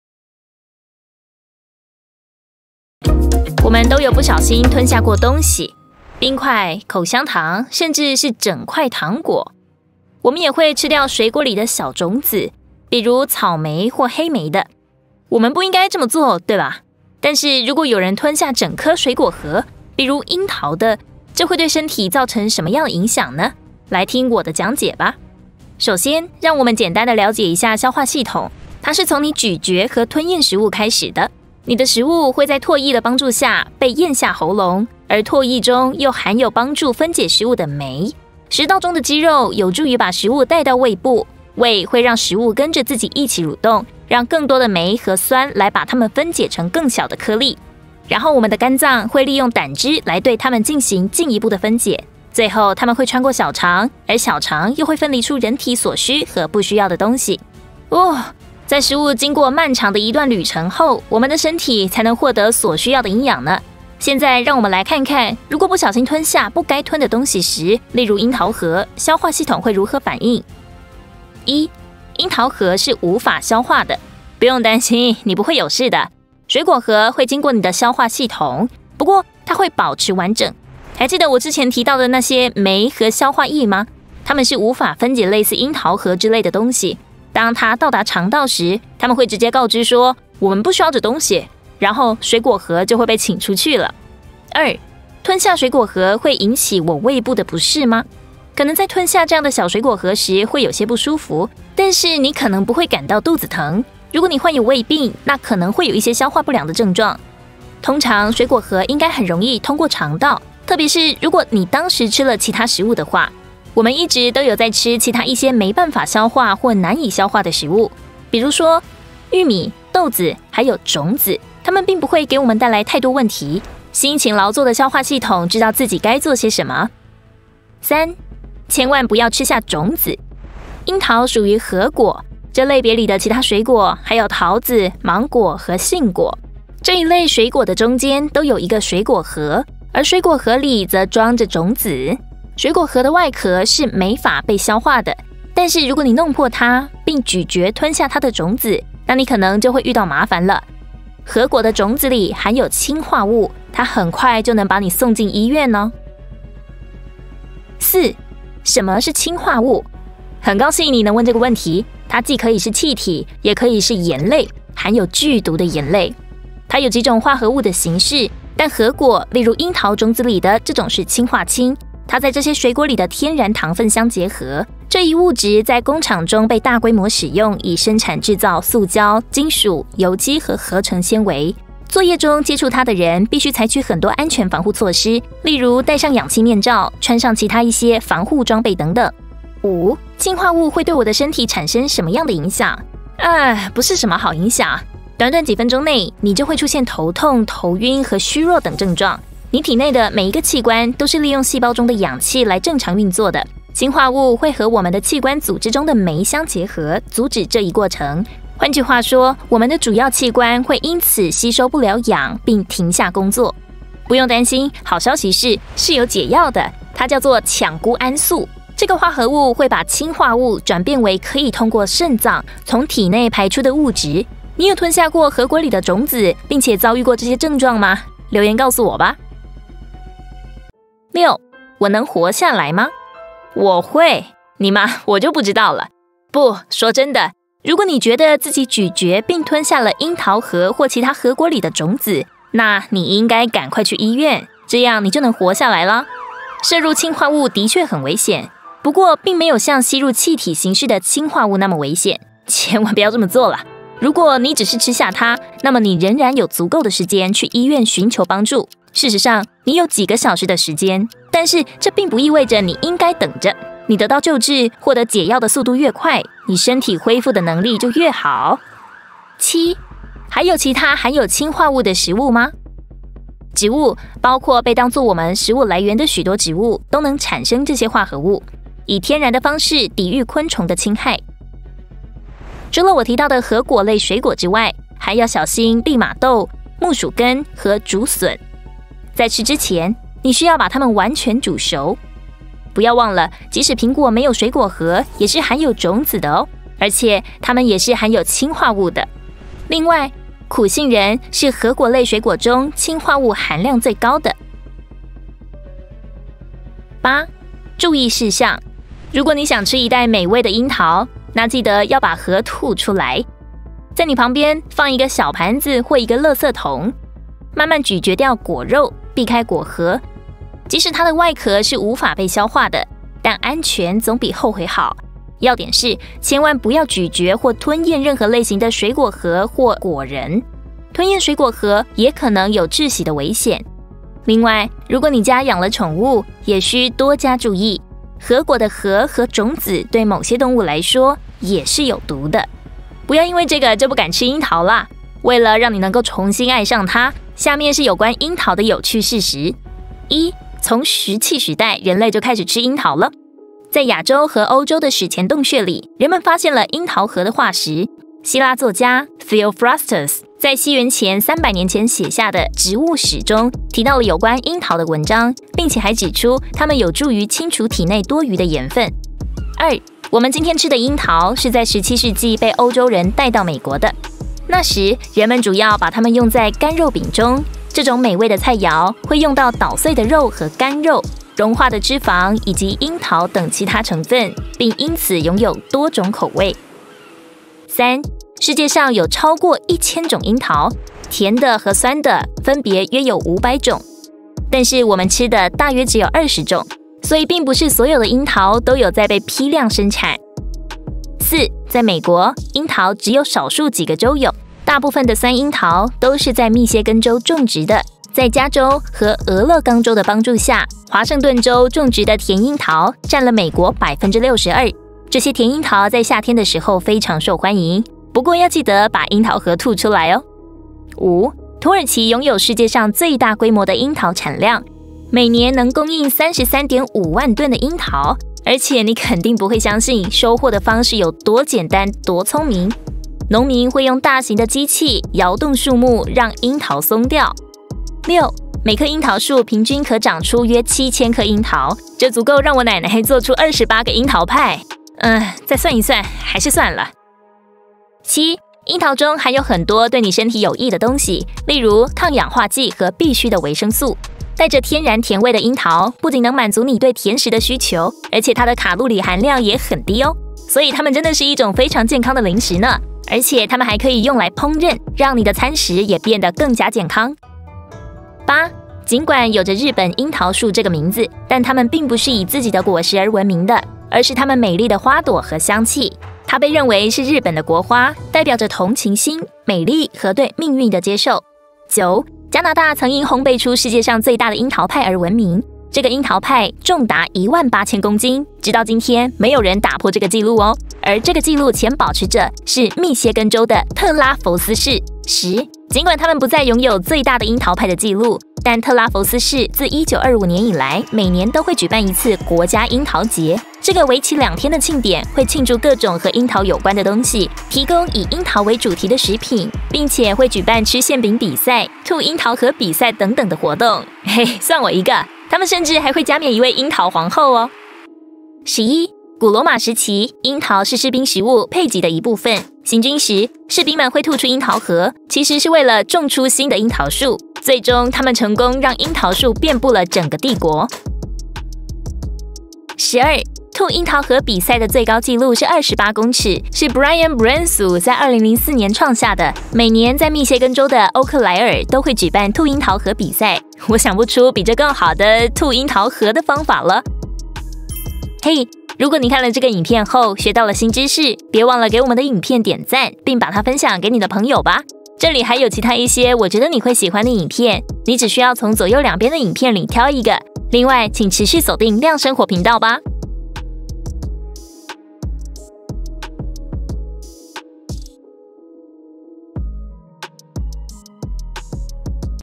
如果你不小心吞下了櫻桃核，會發生什麼呢？
如果你不小心吞下了櫻桃核，會發生什麼呢？吞下櫻桃核安全嗎？我們都有不小心吞下過東西——冰塊、口香糖、甚至是整塊糖果。我們也會吃掉水果裡的小種子，比如草莓或黑莓的。但是，如果人吞下整顆水果核，這會對身體造成什麼樣的影響呢？

對了，櫻桃屬於核果。這一類的水果的中間都有一個水果核，而水果核裡則裝著種子。如果你弄破它，並咀嚼吞下它的種子，那你可能就會遇到麻煩了。核果的種子裡是含有氰化物的喔！

時間戳:
櫻桃核是無法消化的 1:11

吞下水果核會引起我胃部的不適嗎？ 1:34

千萬不要吃下種子！ 2:08

什麼是氰化物？ 2:38

氰化物會對身體產生什麼樣的影響？ 3:10

我能活下來嗎？3:51

還有其他含有氰化物的食物嗎？ 4:30

注意事項 5:07

有趣事實 5:49

123 GO! Chinese

123 GO! Challenge Chinese

123 GO! Gold Chinese